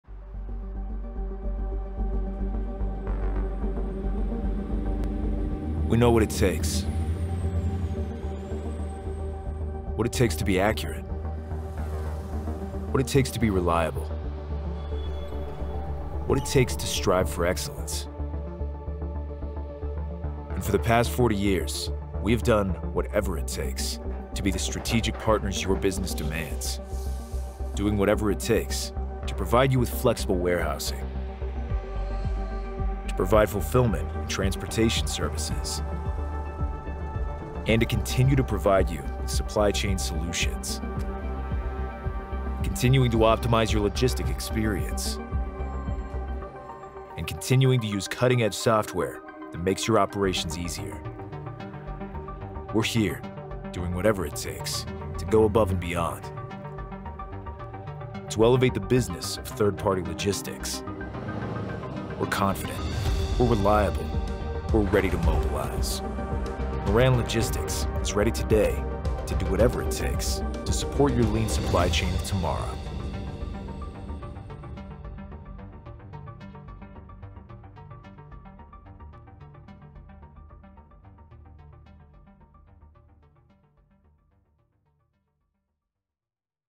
Moran Logistics "Whatever It Takes" Commercial
With over 40 years of experience, our American dream has always hinged upon the shared success of our partners.

Offering over 5 million square feet of premier warehouse space in locations across Pennsylvania, Moran Logistics is confident and ready to mobilize – to do Whatever It Takes to meet your needs and provide you with the supply chain solutions your business demands.

With strategically located facilities in the heart of the northeast distribution market, we’re ready to streamline your supply chain with flexible warehousing, fulfillment and transportation services that allow you to reach 60% of North American consumers in an 8 hours drive or less.

Our professional services are here to optimize your logistic experience and make operations easier and simpler than ever before via our cutting-edge tier-one warehouse and transportation management software solutions. Partnership in our industry demands accuracy, reliability and a level of excellence that makes complex logistic challenges simple.

We’re here, going above and beyond to elevate the business of third-party logistics to a new level. Let Moran Logistics do Whatever It Takes to service your lean supply chain of tomorrow.